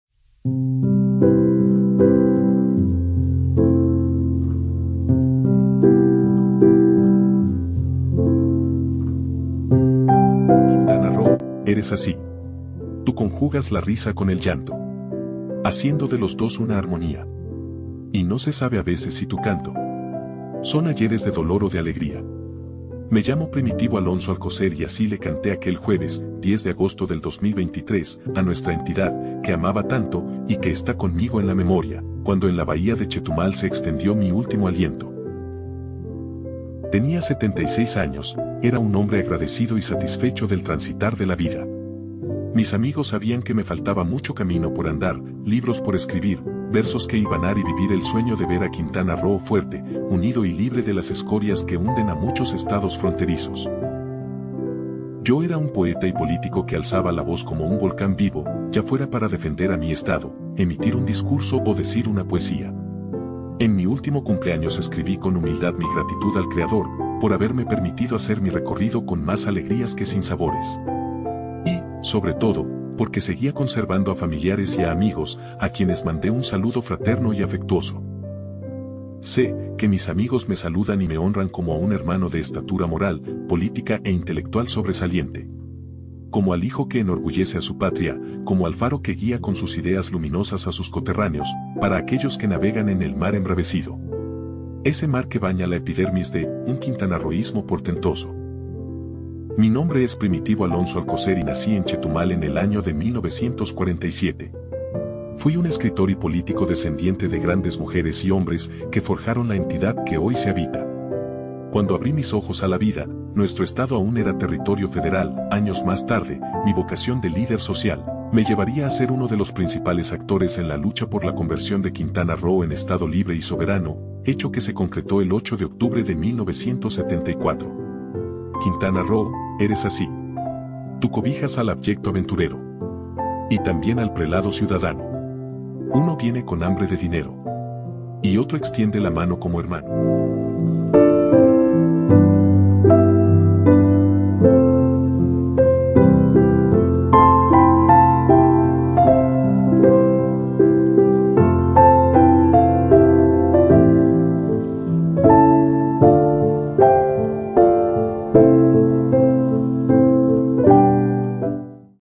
PASILLO HISTÓRICO UNICARIBE - Primitivo Alonso
Como parte del 50 Aniversario del Estado de Quintana Roo se presenta el Pasillo Histórico que alberga biografías y videos de los principales personajes históricos de este Estado.